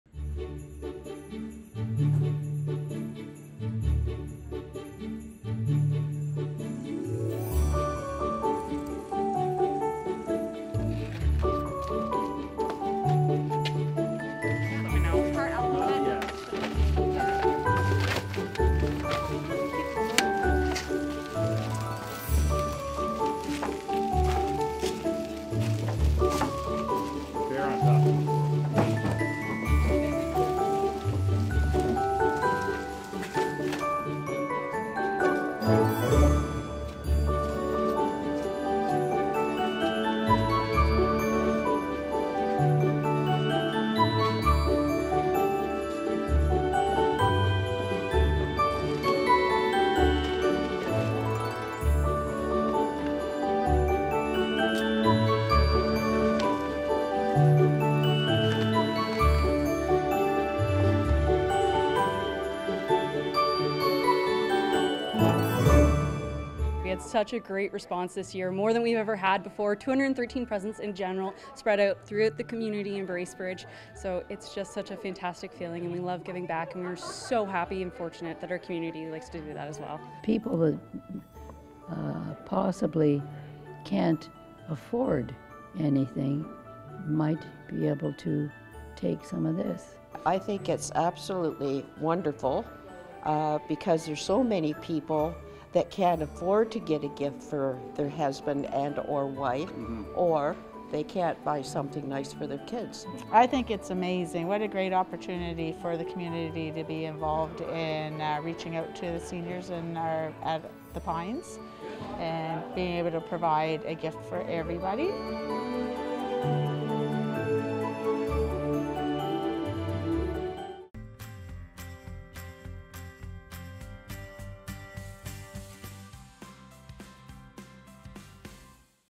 The Pines Secret Santa Senior Gift Drop Off.
Earlier this month we told you about the Bracebridge Public Library's Secret Santa program, partnering community members with seniors at care homes across the municipality. Well, Bracebridge, you really got into the spirit!